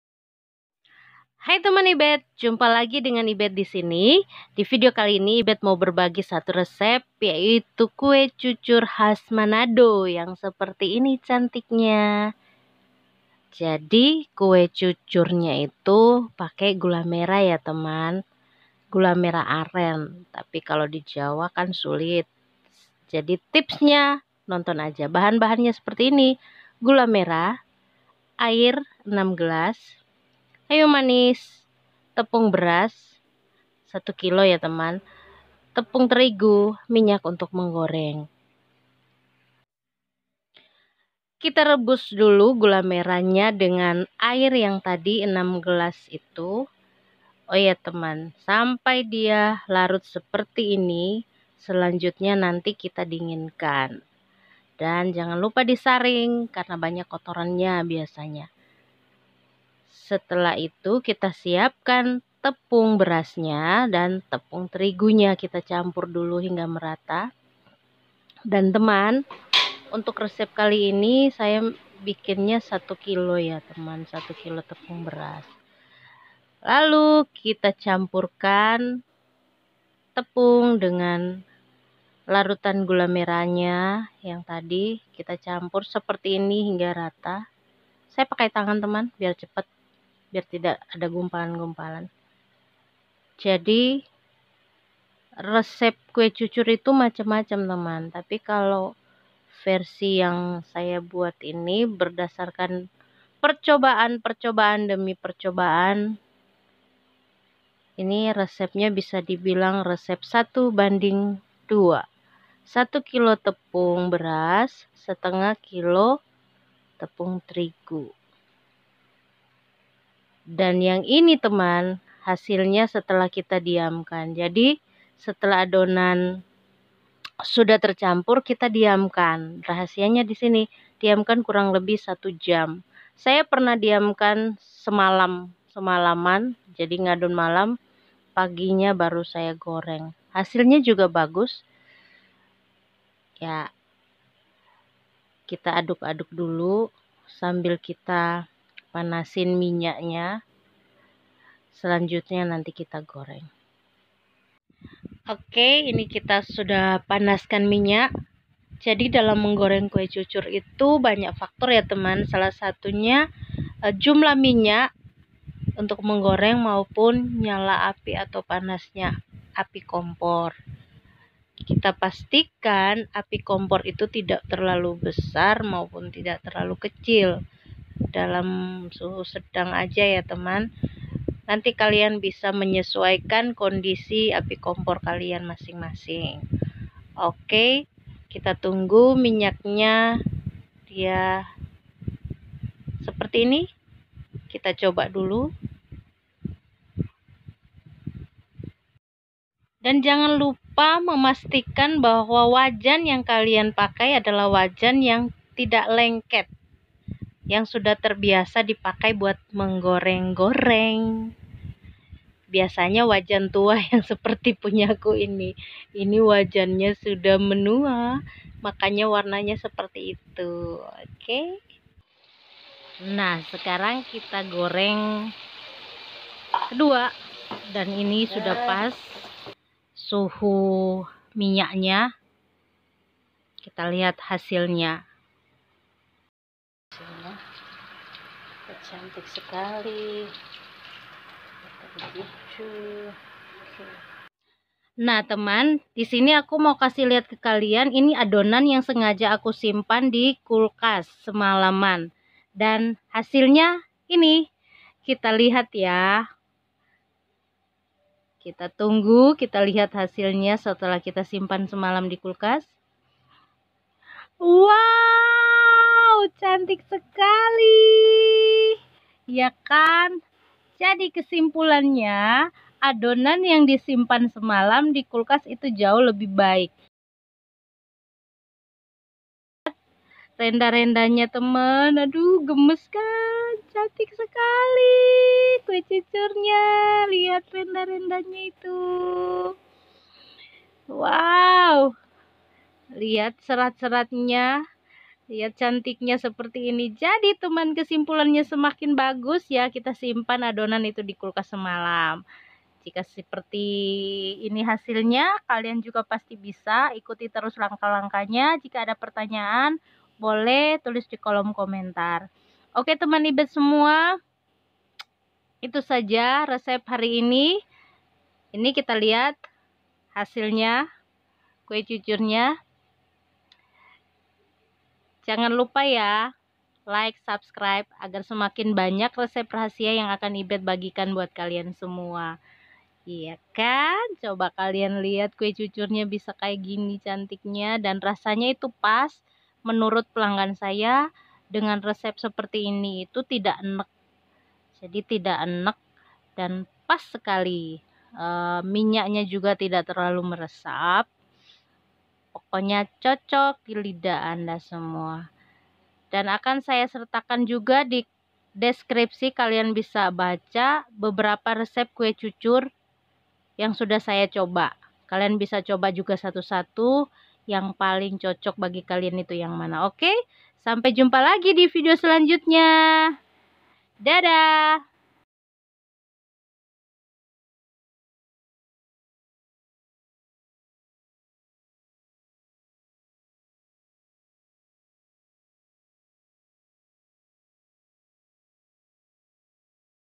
RESEP KUE CUCUR ASLI MANADO /KHAS SANGER SIAU
Tiap daerah pasti ada jajanan yg satu ini kue cucur klo di kampungku sanger,manado,siau.di tempat kaluan namanya apa teman? Semoga resep rahasia by ibeth's cake ini bermanfaat buat teman semua,dan di deskripsi ini ibeth sertakan beberapa resepnya
RESEPNYA :
 A)RESEP A
 1KG TEPUNG BERAS
500GR TEPUNG TERIGU
6  GELAS AIR(1200ML)
1KG GULA MERAH
1 sdm gula pasir
garam (bisa di skip)

B) RESEP B
1KG TEPUNG BERAS
750GR TEPUNG TERIGU
7 GELAS AIR (1400ML)
1KG GULA MERAH
100GR GULA PASIR
KOCOK SEMUA ADONAN HINGGA TERCAMPUR RATA,DIAMKAN 1 JAM SETELAH ITU ADONAN SIAP DI GORENG